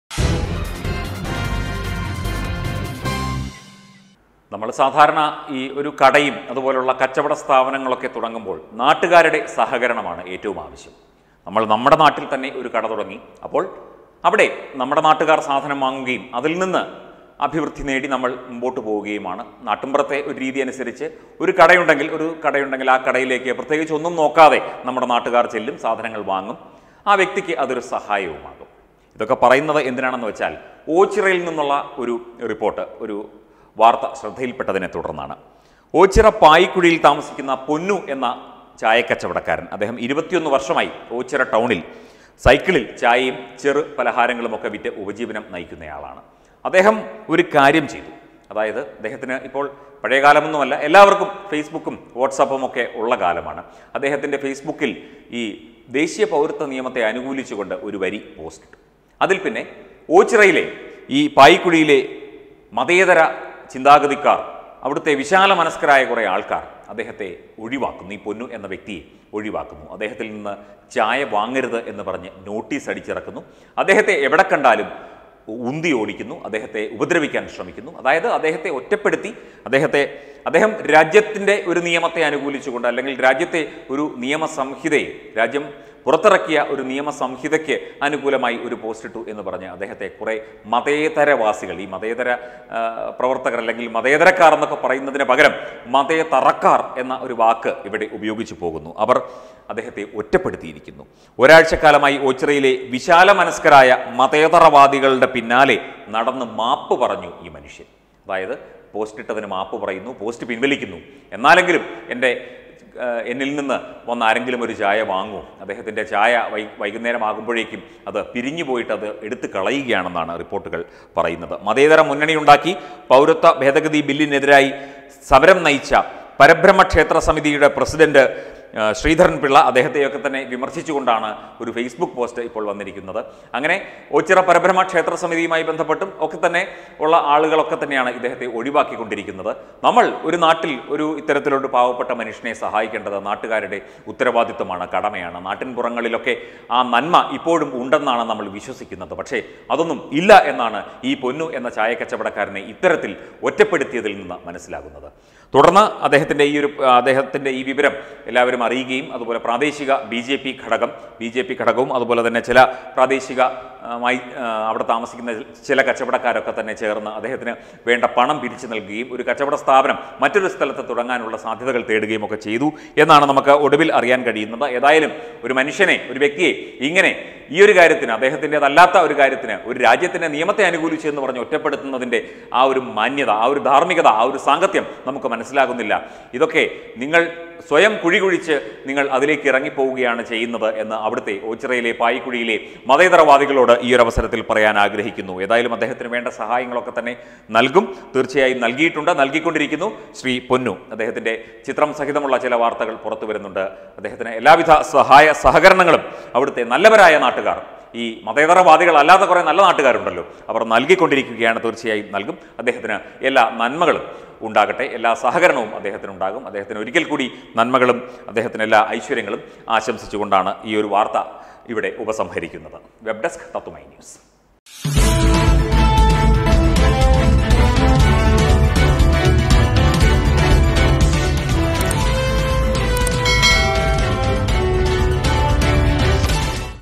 ഒരാളെ ജീവിക്കാൻ സമ്മതിക്കാതെ മതേതരവാദികൾ...
ഇതെന്തൊരു നാടാണ്,രാഷ്ട്രം പാസാക്കിയ നിയമത്തെ അനുകൂലിച്ച ഒരു പാവത്തിന്റെ ചായക്കച്ചവടം പൂട്ടിച്ച മതേതരവാദികളെ,ലജ്ജ തോന്നുന്നു നിങ്ങളെയോർത്ത്...